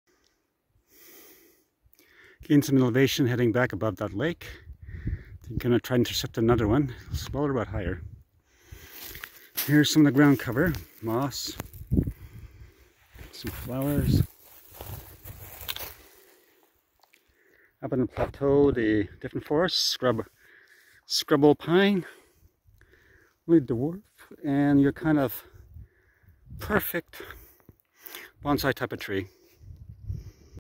Walk across Scrub Pine May.20.23
Walk up from the lake to take shorter route return to Shearwater.